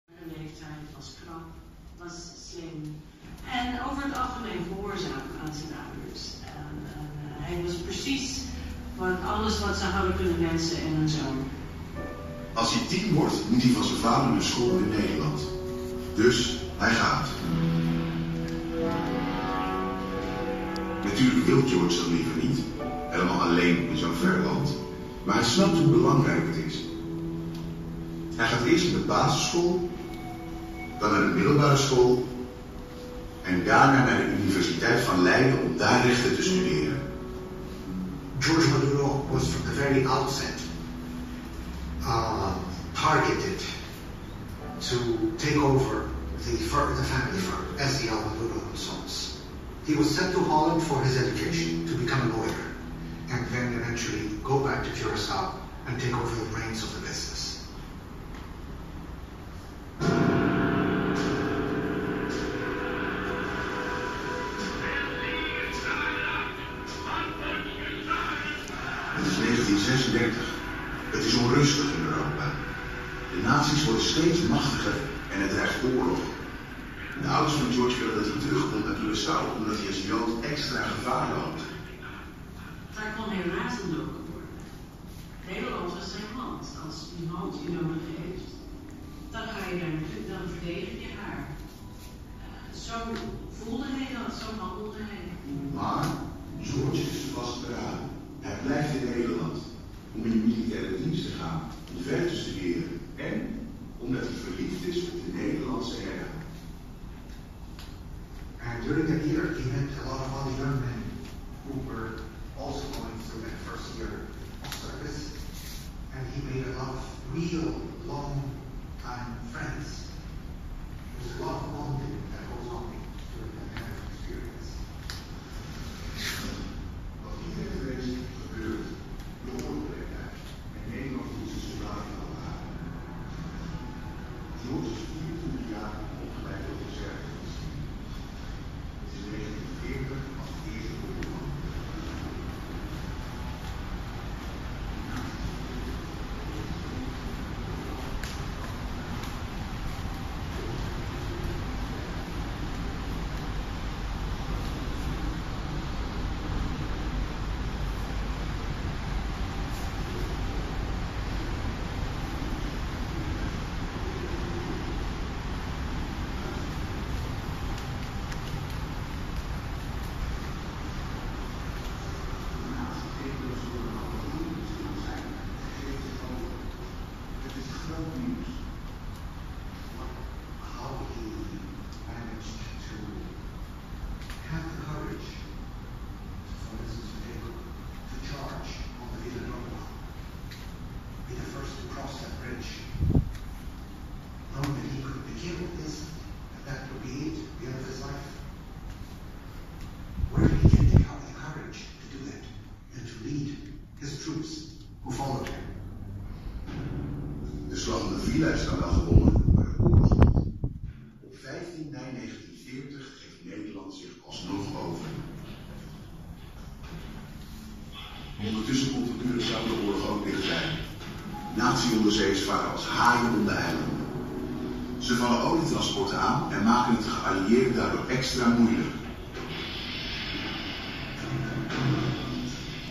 Tentoonstelling George Maduro Curacao
Impressie van de tentoonstelling George Maduro in de Biblioteka Nashonal Korsou 24 aug-31 dec 2016.